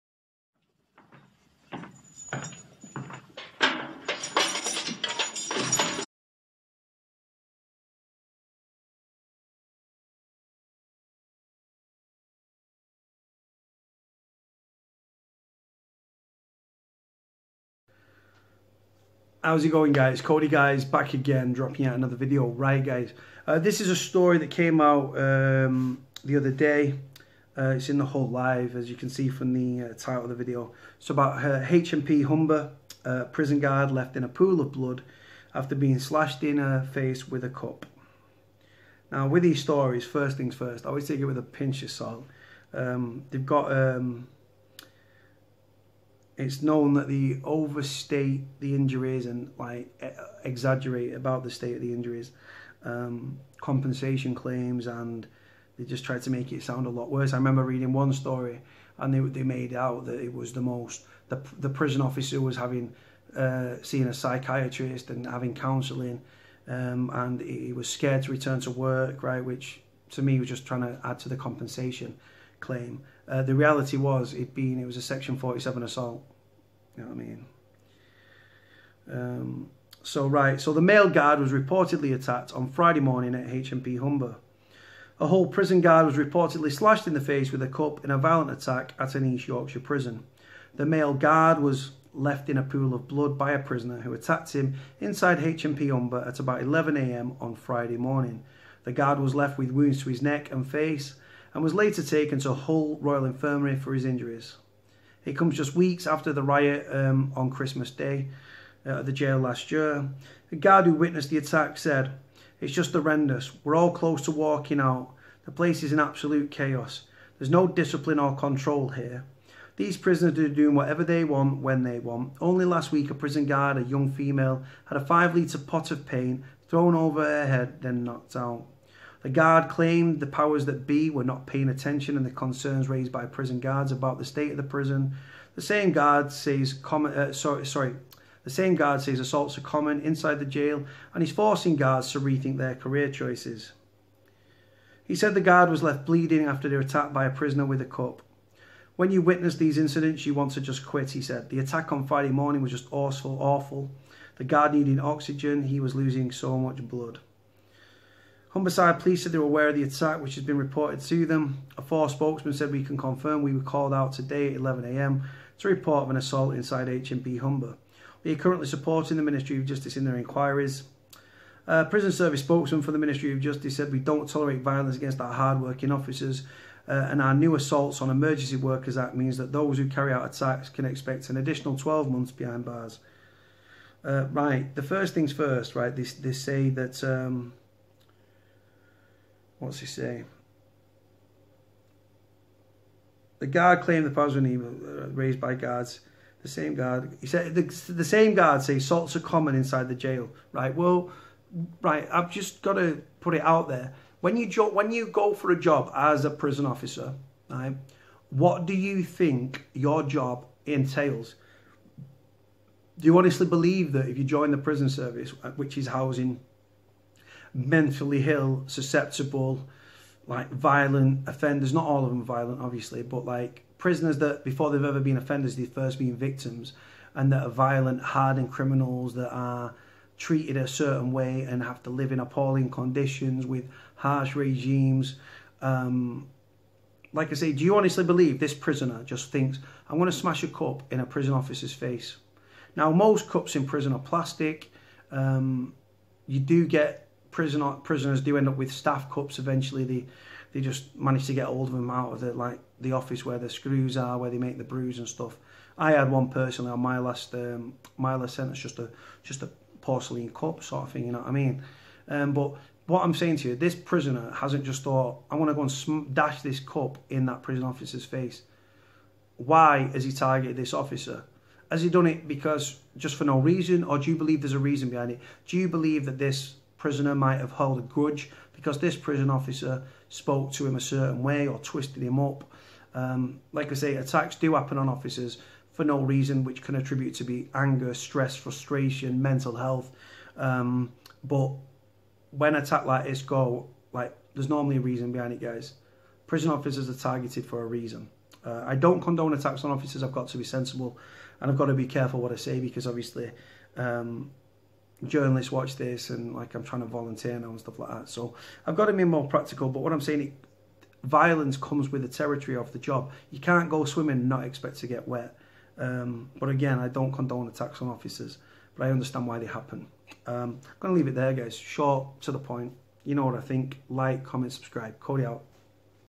HMP Humber prison guard 'left in pool of blood' after being 'slashed in face with cup'
My latest vlog this time on HMP Humber prison guard 'left in pool of blood' after being slashed in the face with cup'.